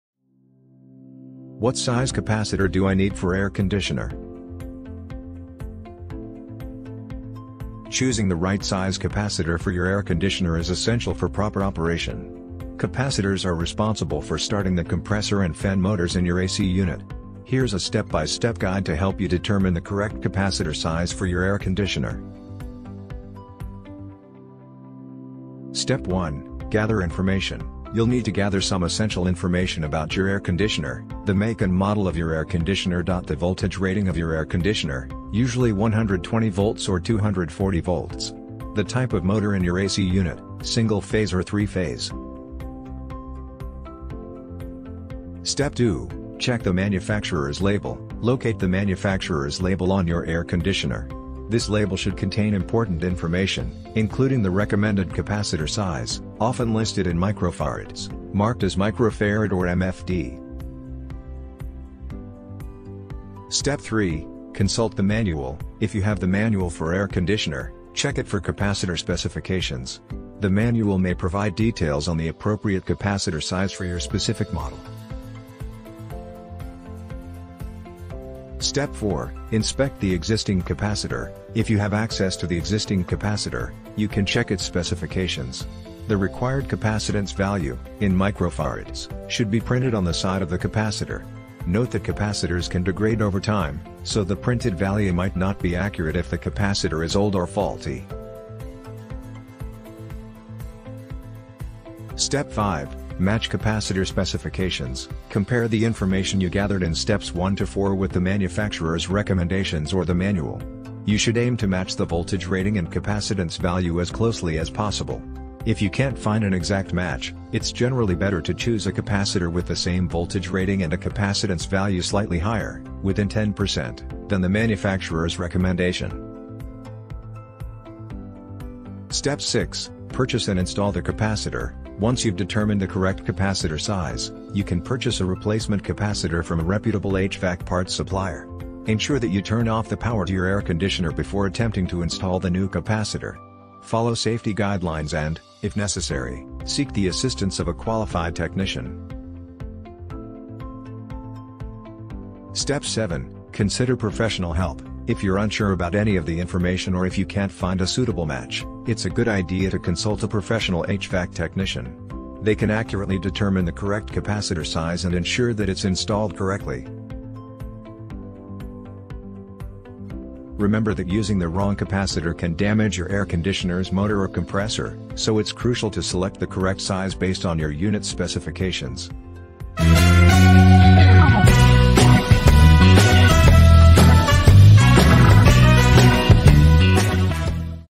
Discover the Right Capacitor Size for Your AC!
The size of the capacitor you need for your air conditioner depends on various factors, including the AC unit's specifications, make, and model. To determine the correct size, refer to your air conditioner's user manual or consult a professional HVAC technician for accurate guidance.